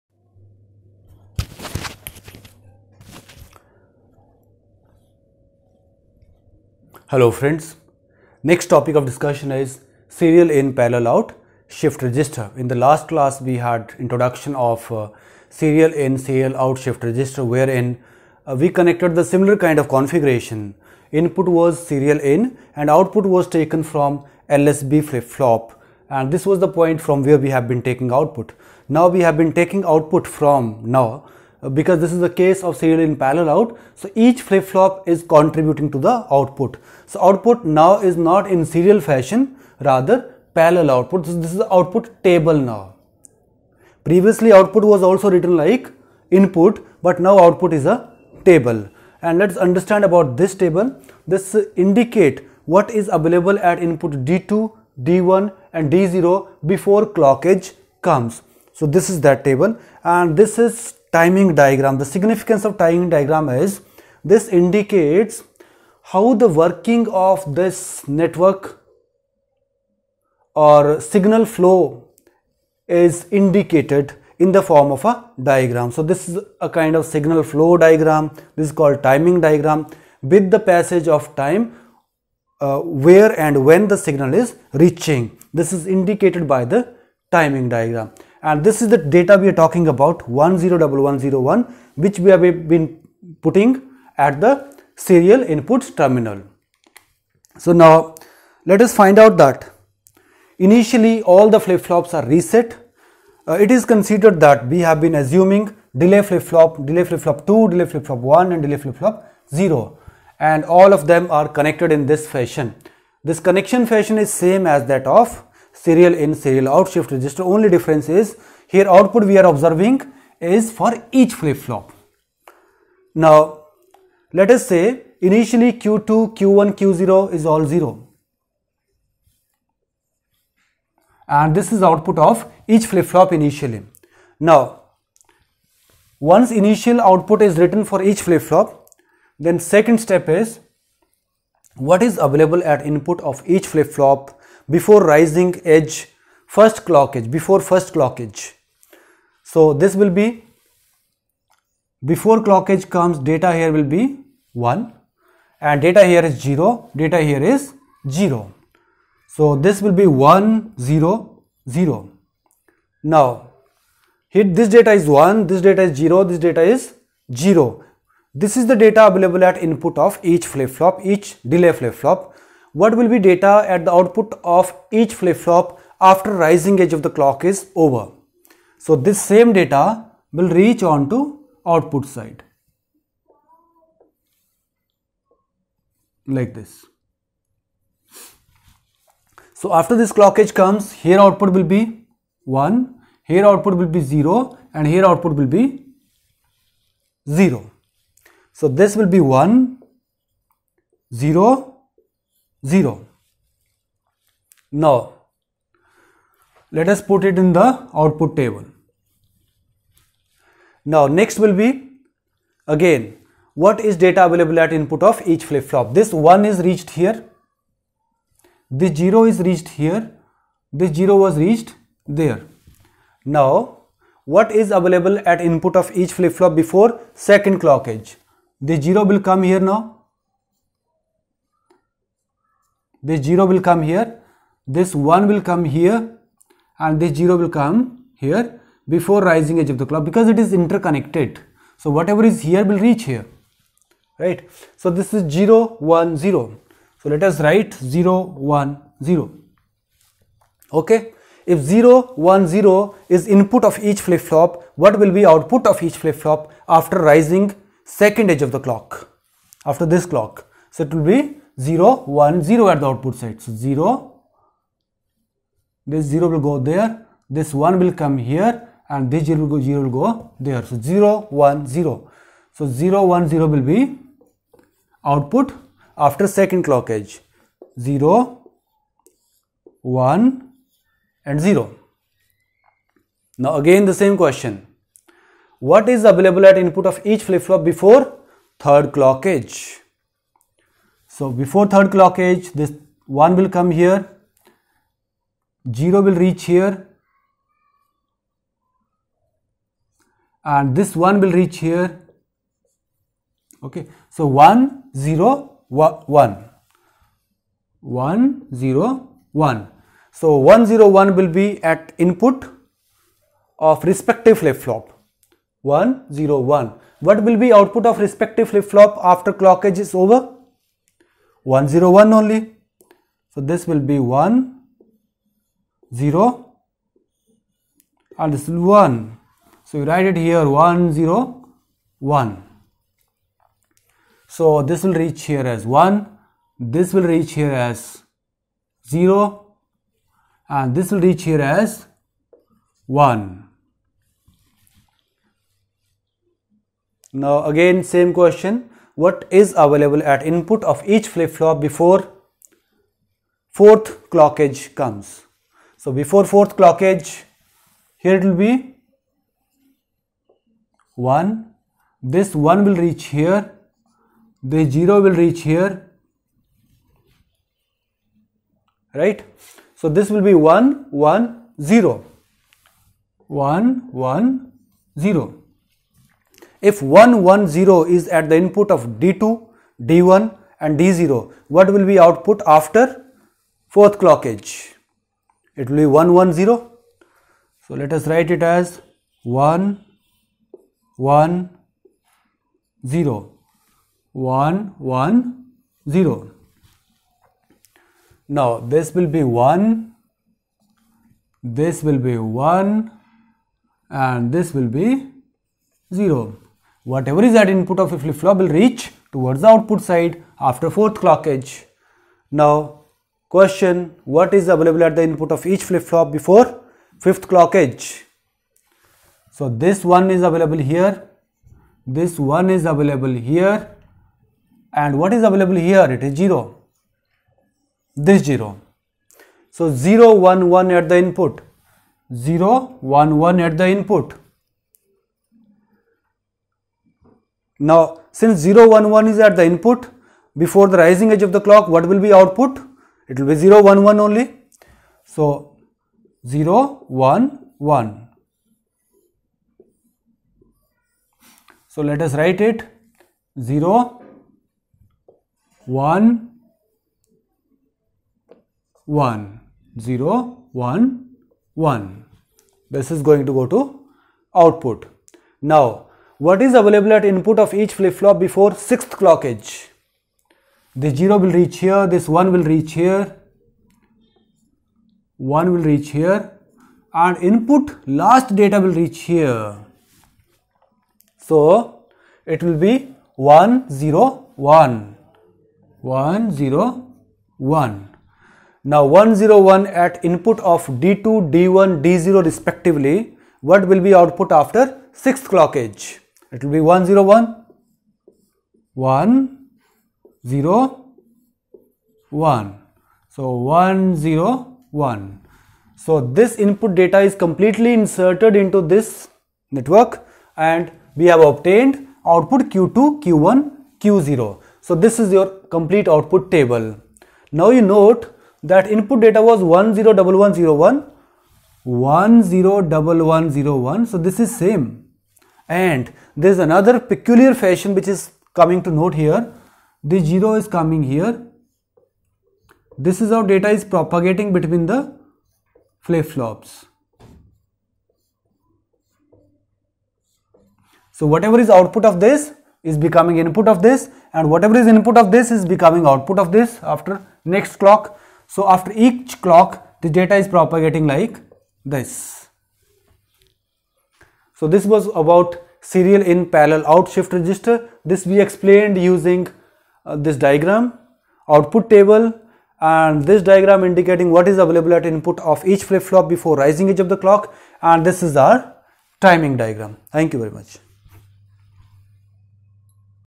Lecture 8: Serial In Parallel Out Shift Register (SIPO)
The detailed working of Serial in Parallel Out Shift Register is explained in this video.